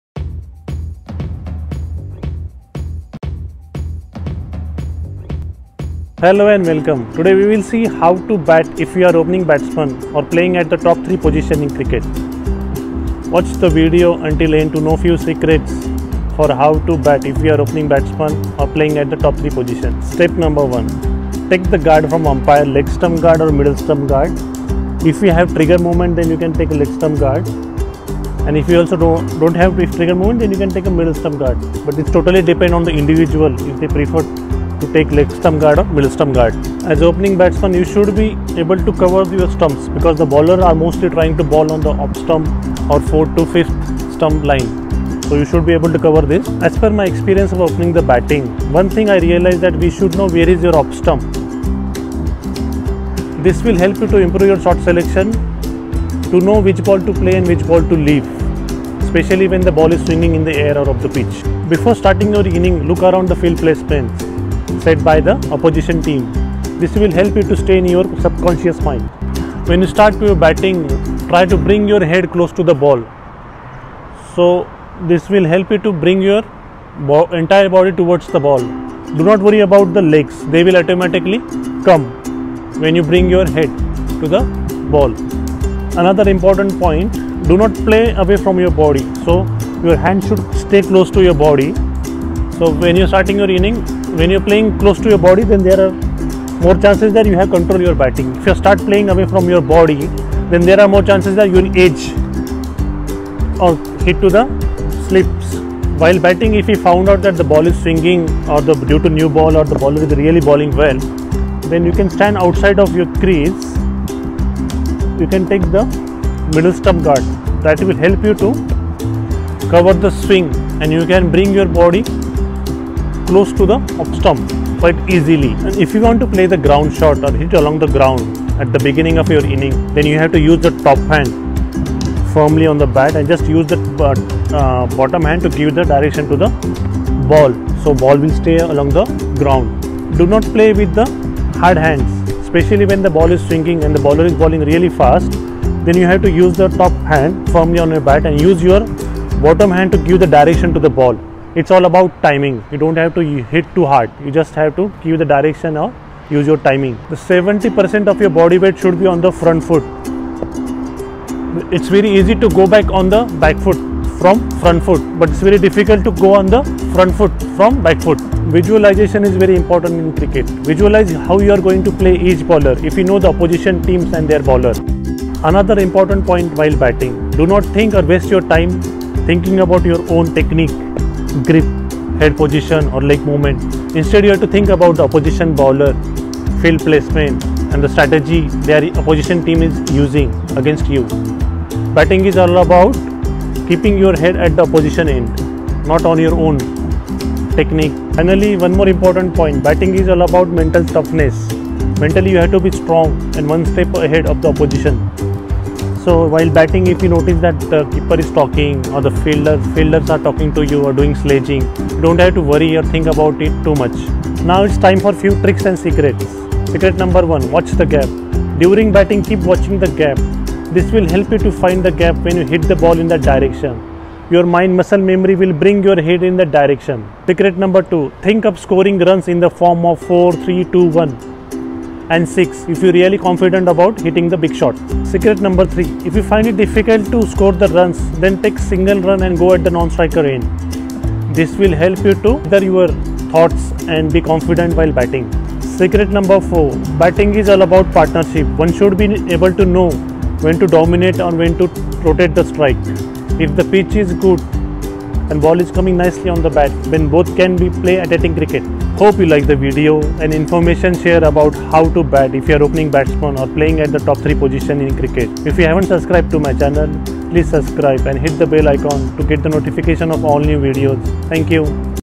How to bat as a opening batsman or playing at top 3 positions in cricket
Hello everyone, today we will see how to bat when you are opening the batting or playing at top 3 position in cricket.

Watch this video until end to know few tricks and secret of opening batting.

Hope you liked the video, please give your comments, and let me know your approach/drills/practice you do when preparing for opening batting.